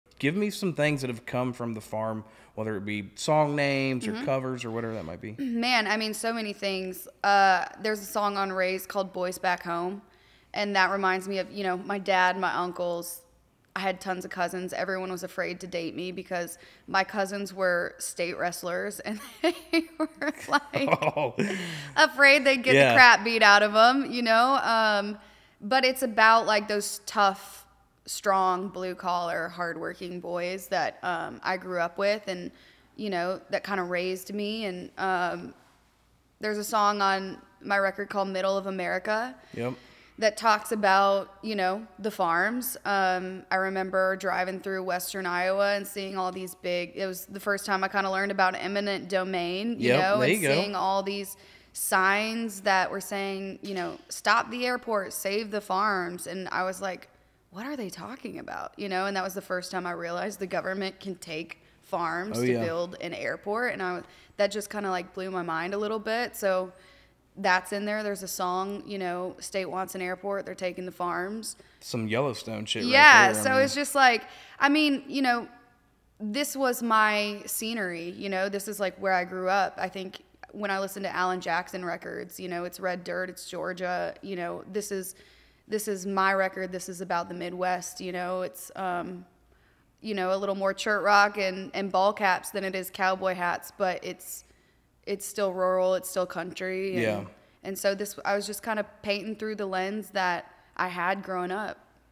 Hailey Whitters: FARMS OVER AIRPORTS!
While airports connect us globally, farms feed us locally. Hailey Whitters discusses being raised in blue collar America, and the importance of preserving our farmland.